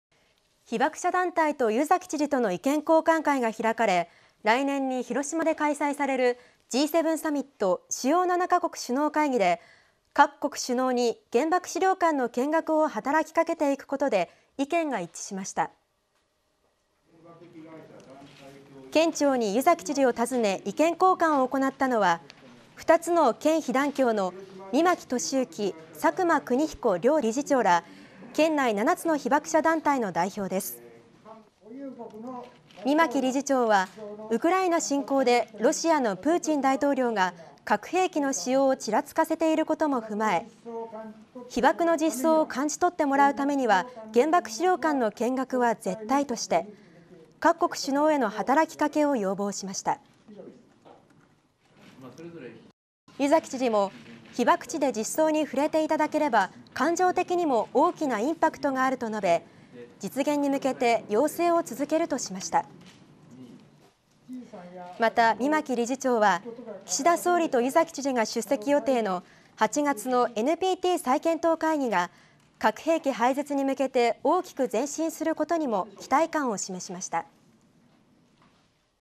【TSSテレビ新広島】湯崎知事 被爆者７団体と意見交換 Governor Yuzaki meets with seven hibakusha organizations
Aired on June 28, 2022　放送日2022/6/28

A meeting was held between Hiroshima Governor Hidehiko Yuzaki and the heads of hibakusha organizations.
At the meeting, all members agreed to work to have the heads of state participating in the G7 Summit visit the Peace Memorial Museum.
Representatives of seven hibakusha organizations, including Chairman Toshiyuki Mimaki and Chairman Kunihiko Sakuma from two Hiroshima-based organizations, held a meeting with the governor.

Based on Vladmir Putin's statements implying the use of nuclear weapons, Chairman Mimaki requested that visiting heads of state tour the museum, stating, "visiting the Peace Memorial Museum is crucial to gaining
an understanding of the realities of the atomic bombing."

Governor Yuzaki stated that learning these realities while in the atomic-bombed city of Hiroshima would also have a huge emotional impact, and said he would work to make a visit to the museum a reality.

Chairman Mimaki also expressed his hopes that the upcoming NPT Review Conference in August will bring about dramatic progress for nuclear weapons abolition.
Both Governor Yuzaki and the Prime Minister of Japan, Fumio Kishida, will be in attendance at the conference.

被爆者団体と湯崎知事との意見交換会が開かれ、来年に広島で開催されるＧ７サミット＝主要７カ国首脳会議で各国首脳に原爆資料館の見学を働きかけていくことで意見が一致しました。
県庁に湯崎知事を訪ね、意見交換を行ったのは、２つの県被団協の箕牧智之、佐久間邦彦両理事長ら県内７つの被爆者団体の代表です。

箕牧理事長はウクライナ侵攻でロシアのプーチン大統領が核兵器の使用をちらつかせていることも踏まえ、「被爆の実相を感じ取ってもらうためには原爆資料館の見学は絶対」として、各国首脳への働きかけを要望しました。

湯崎知事も「被爆地で実相に触れていただければ感情的にも大きなインパクトがある」と述べ、実現に向けて要請を続けるとしました。また、箕牧理事長は岸田首相と湯崎知事が出席予定の８月のＮＰＴ再検討会議が核兵器廃絶に向けて大きく前進することにも期待感を示しました。